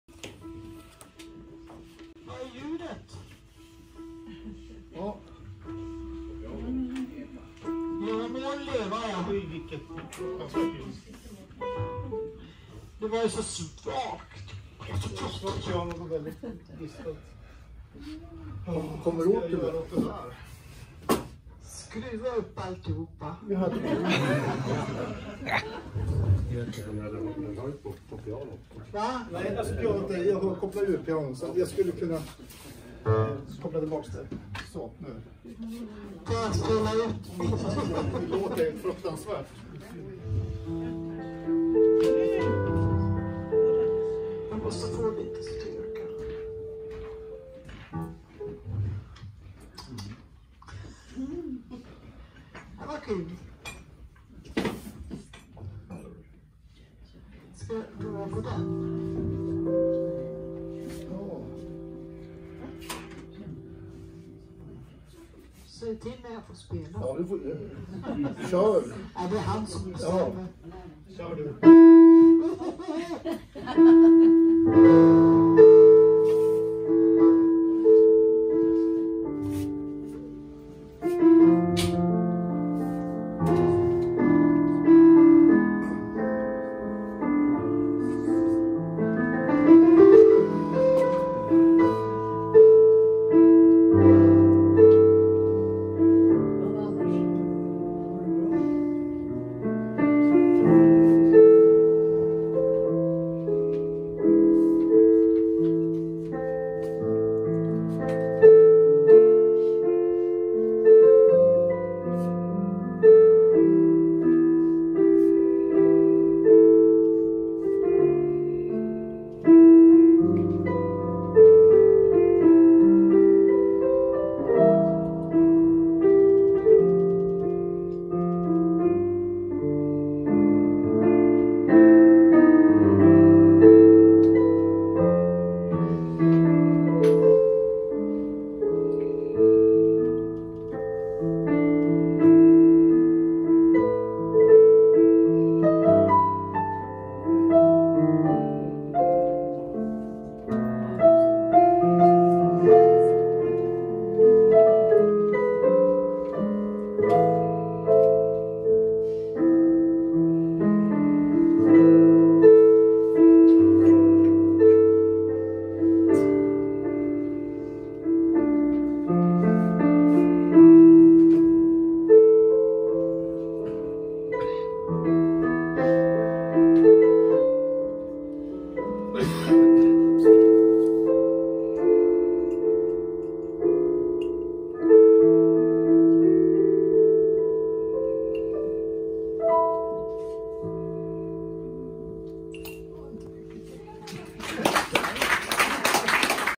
Umeå Live - Birgit Lindberg
Birgit Lindberg på Bokcafé Pilgatan, feb 2022.